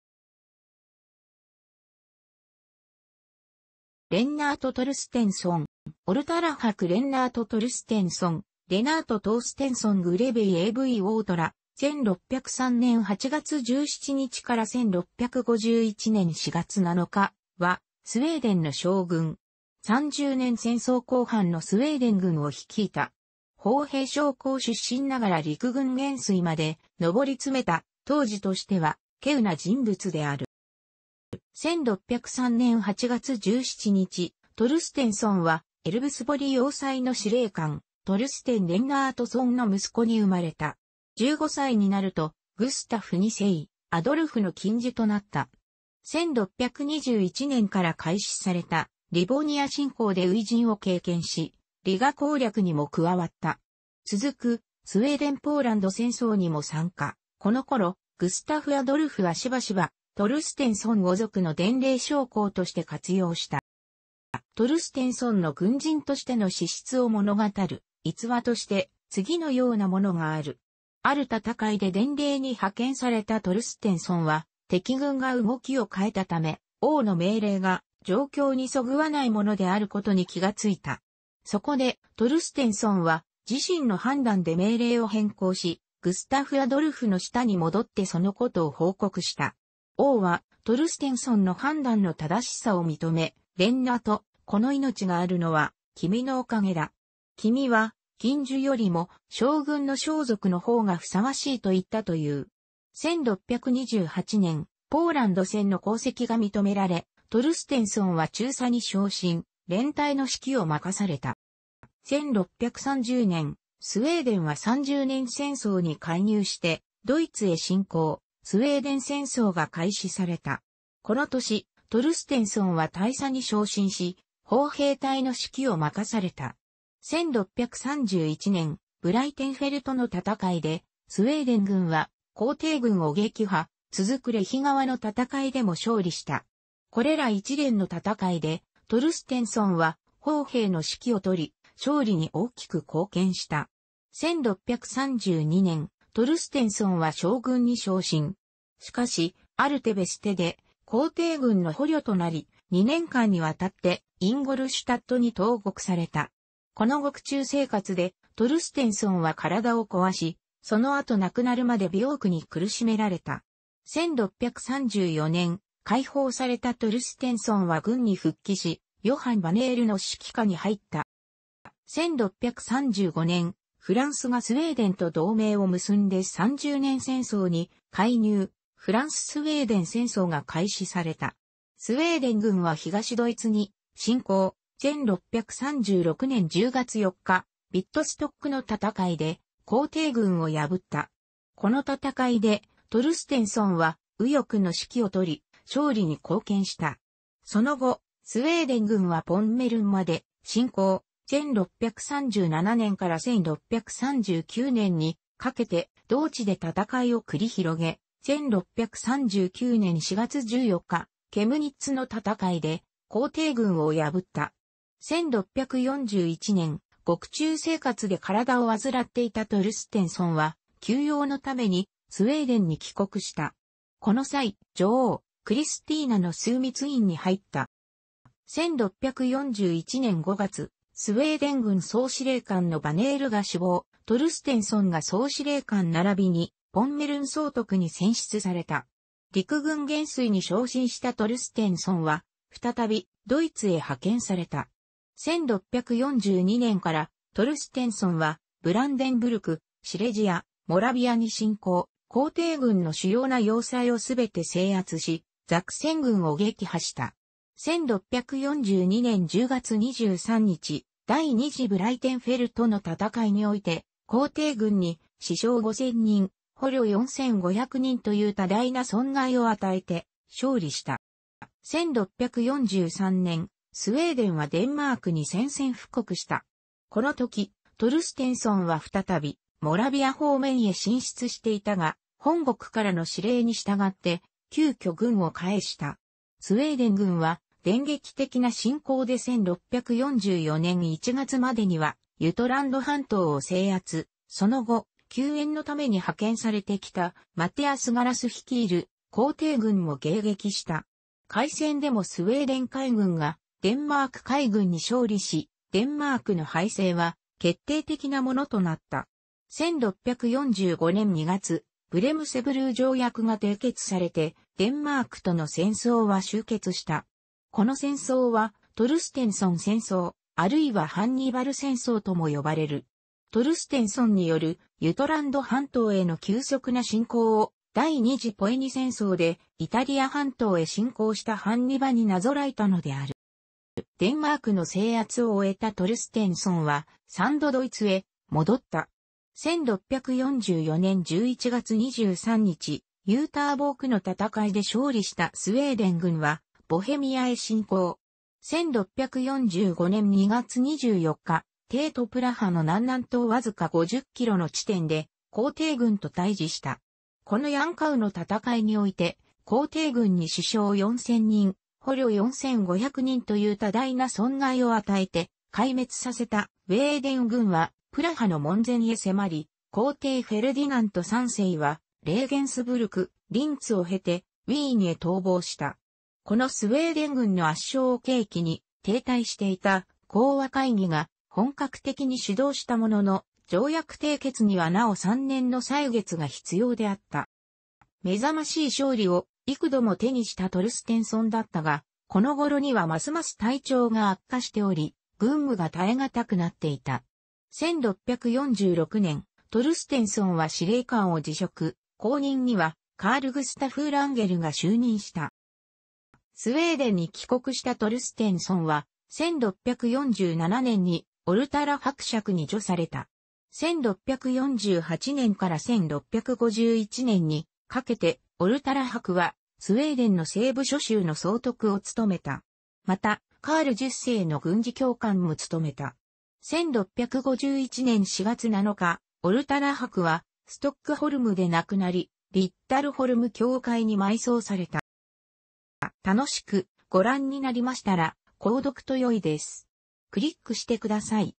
レンナート・トルステンソン
オルタラ伯レンナート・トルステンソン（Lennart Torstenson Greve av Ortala、1603年8月17日 - 1651年4月7日）は、スウェーデンの将軍。三十年戦争後半のスウェーデン軍を率いた。砲兵将校出身ながら陸軍元帥まで上り詰めた、当時としては稀有な人物である。

1603年8月17日、トルステンソンはエルヴスボリ要塞の司令官トルステン・レンナートソンの息子に生まれた。15歳になるとグスタフ2世アドルフの近習となった。1621年から開始されたリヴォニア侵攻で初陣を経験し、リガ攻略にも加わった。続くスウェーデン・ポーランド戦争にも参加、この頃、グスタフ・アドルフはしばしばトルステンソンを直属の伝令将校として活用した。

トルステンソンの軍人としての資質を物語る逸話として次のようなものがある。ある戦いで伝令に派遣されたトルステンソンは、敵軍が動きを変えたため、王の命令が状況にそぐわないものであることに気がついた。そこでトルステンソンは自身の判断で命令を変更し、グスタフ・アドルフの下に戻ってそのことを報告した。王はトルステンソンの判断の正しさを認め、「レンナート、この命があるのは君のおかげだ。君は近習よりも将軍の装束の方が相応しい」と言ったという。1628年、ポーランド戦の功績が認められ、トルステンソンは中佐に昇進、連隊の指揮を任された。

1630年、スウェーデンは三十年戦争に介入してドイツへ侵攻、スウェーデン戦争が開始された。この年、トルステンソンは大佐に昇進し、砲兵隊の指揮を任された。1631年、ブライテンフェルトの戦いでスウェーデン軍は皇帝軍を撃破、続くレヒ川の戦いでも勝利した。これら一連の戦いでトルステンソンは砲兵の指揮を執り、勝利に大きく貢献した。1632年、トルステンソンは将軍に昇進。しかし、アルテ・ベステで皇帝軍の捕虜となり、2年間にわたってインゴルシュタットに投獄された。この獄中生活でトルステンソンは体を壊し、その後亡くなるまで病躯に苦しめられた。1634年、開放されたトルステンソンは軍に復帰し、ヨハン・バネールの指揮下に入った。

1635年、フランスがスウェーデンと同盟を結んで三十年戦争に介入、フランス・スウェーデン戦争が開始された。スウェーデン軍は東ドイツに侵攻、1636年10月4日、ヴィットストックの戦いで皇帝軍を破った。この戦いでトルステンソンは右翼の指揮を執り、勝利に貢献した。その後、スウェーデン軍はポンメルンまで侵攻、1637年から1639年にかけて同地で戦いを繰り広げ、1639年4月14日、ケムニッツの戦いで皇帝軍を破った。1641年、獄中生活で体を患っていたトルステンソンは、休養のためにスウェーデンに帰国した。この際、女王クリスティーナの枢密院に入った。

1641年5月、スウェーデン軍総司令官のバネールが死亡、トルステンソンが総司令官ならびにポンメルン総督に選出された。陸軍元帥に昇進したトルステンソンは、再びドイツへ派遣された。1642年からトルステンソンはブランデンブルク、シレジア、モラヴィアに侵攻、皇帝軍の主要な要塞をすべて制圧し、ザクセン軍を撃破した。1642年10月23日、第二次ブライテンフェルトの戦いにおいて、皇帝軍に死傷5,000人、捕虜4,500人という多大な損害を与えて勝利した。

1643年、スウェーデンはデンマークに宣戦布告した。この時、トルステンソンは再びモラヴィア方面へ進出していたが、本国からの指令に従って急遽軍を返した。スウェーデン軍は電撃的な侵攻で1644年1月までにはユトランド半島を制圧、その後、救援のために派遣されてきたマティアス・ガラス率いる皇帝軍も迎撃した。海戦でもスウェーデン海軍がデンマーク海軍に勝利し、デンマークの敗勢は決定的なものとなった。1645年2月、ブレムセブルー条約が締結されてデンマークとの戦争は終結した。この戦争はトルステンソン戦争、あるいはハンニバル戦争とも呼ばれる。トルステンソンによるユトランド半島への急速な侵攻を、第二次ポエニ戦争でイタリア半島へ侵攻したハンニバルになぞらえたのである。

デンマークの制圧を終えたトルステンソンは、三度ドイツへ戻った。1644年11月23日、ユーターボークの戦いで勝利したスウェーデン軍はボヘミアへ侵攻。1645年2月24日、帝都プラハの南南東わ...